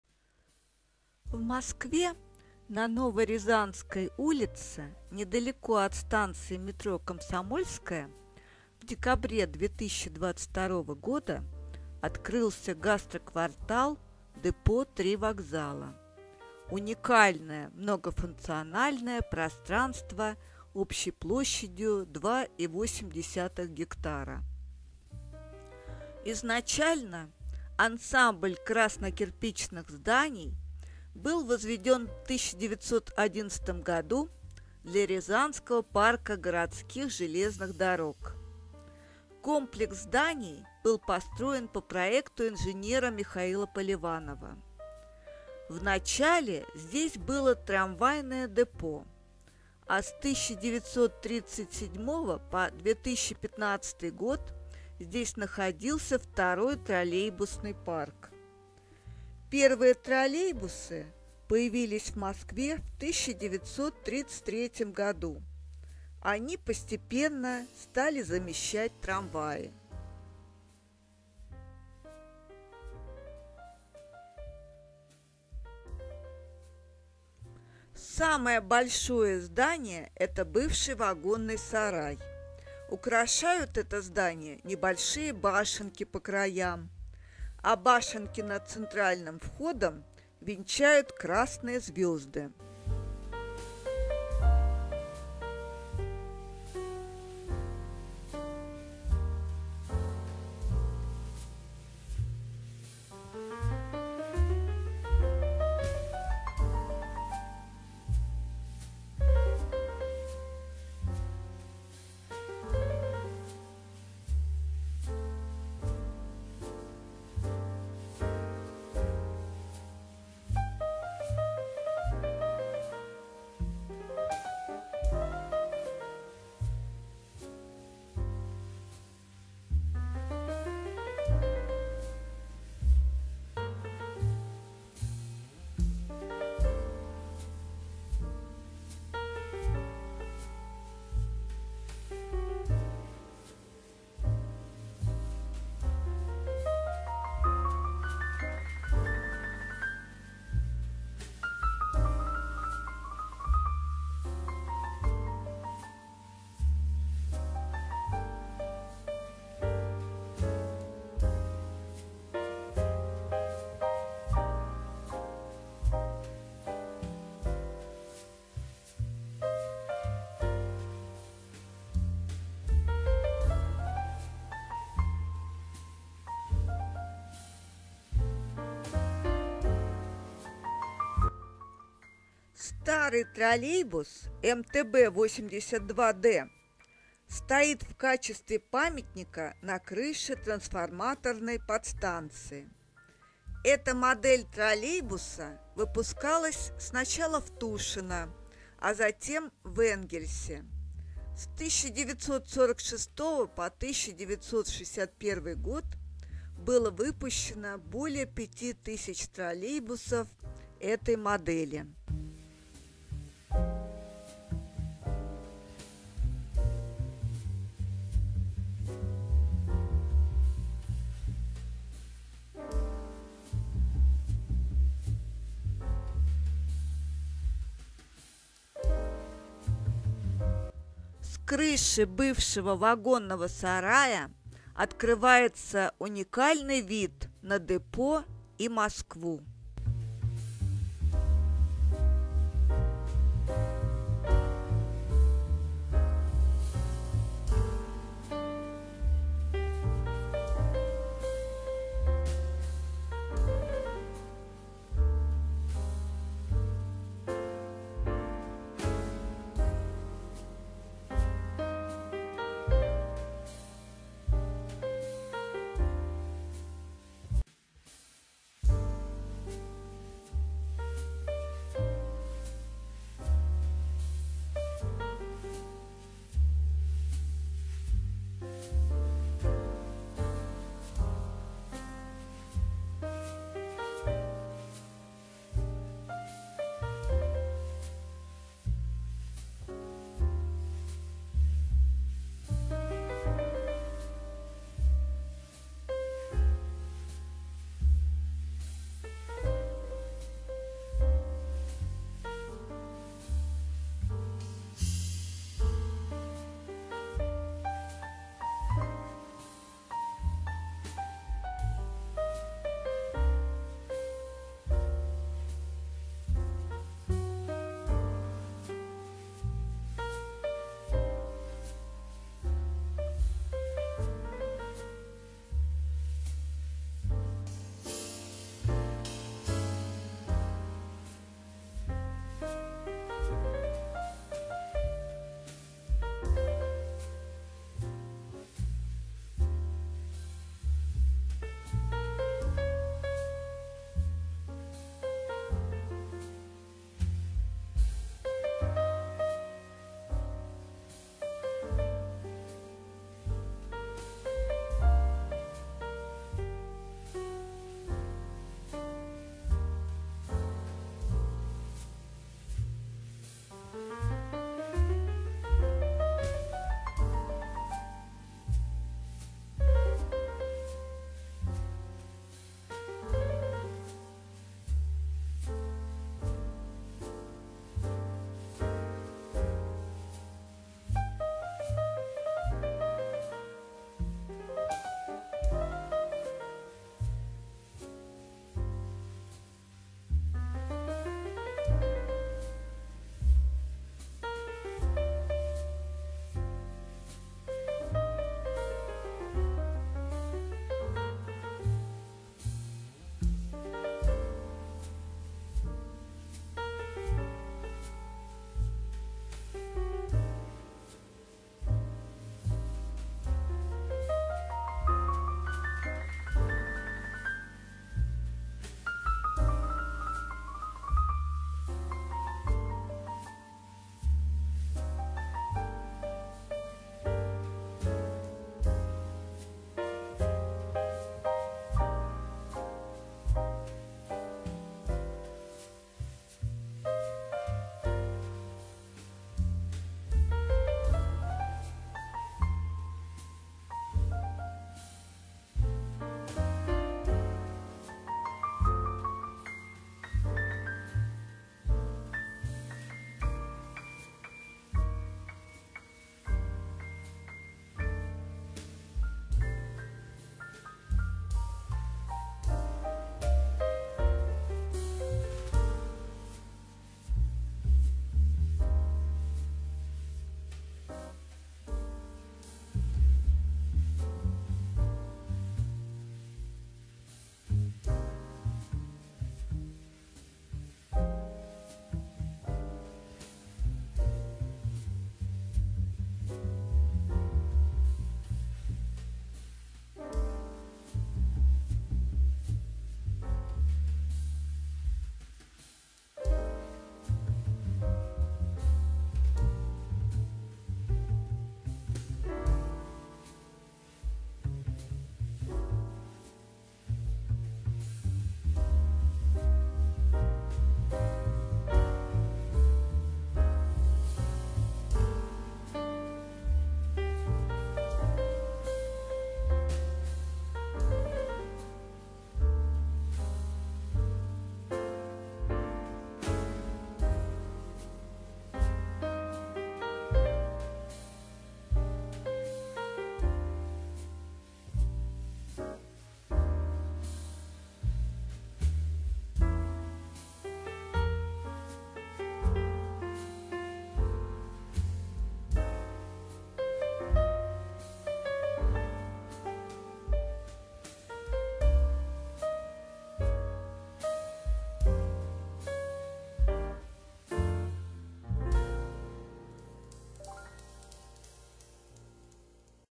Москва 2024. Прогулка по гастрокварталу "Депо Три вокзала" на Новорязанской улице.
Прогулки по Москве